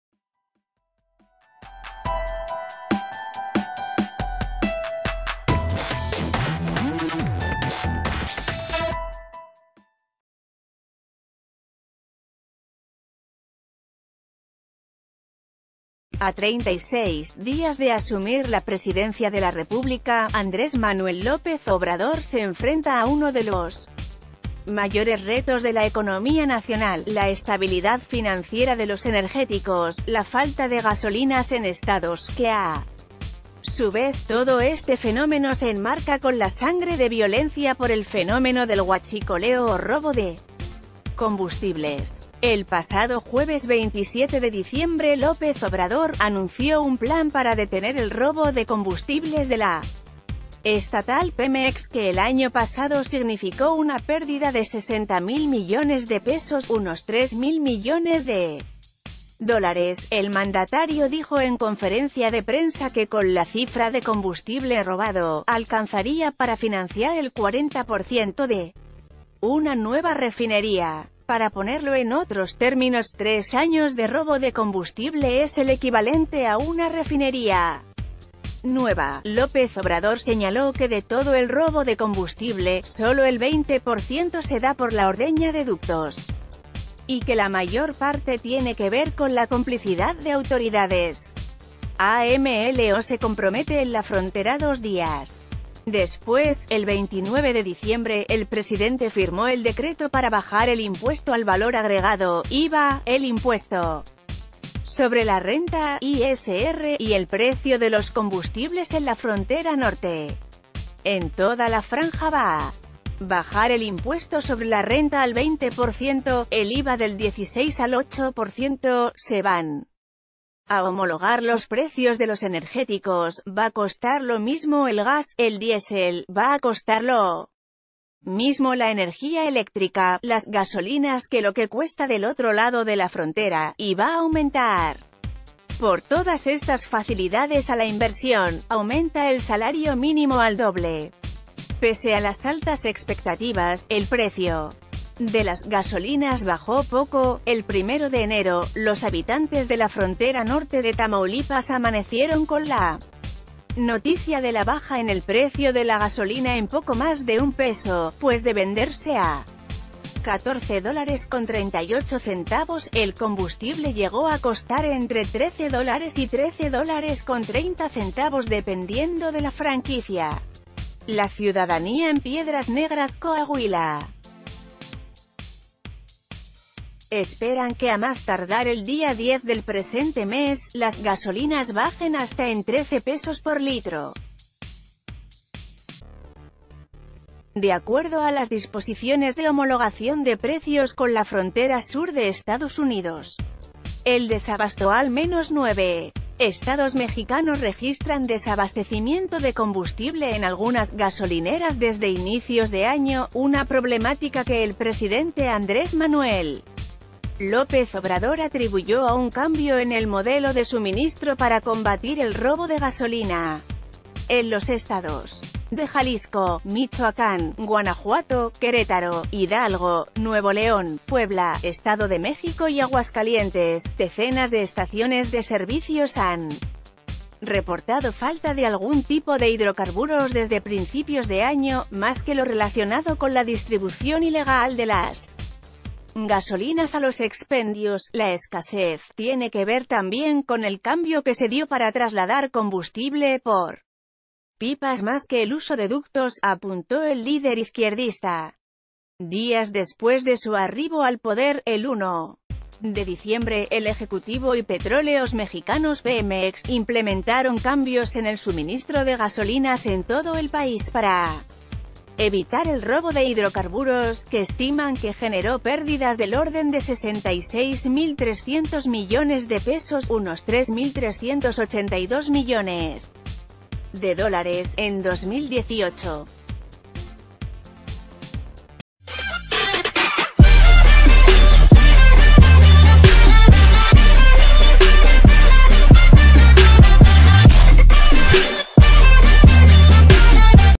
Te explicamos la crisis de combustible que enfrenta AMLO a sólo 5 días de 2019
A 36 días de asumir la presidencia de la República, Andrés Manuel López Obrador se enfrenta a uno de los mayores retos de la economía nacional: la estabilidad financiera de los energéticos, la falta de gasolinas en estados, que a su vez, todo este fenómeno se enmarca con la sangre de violencia por e...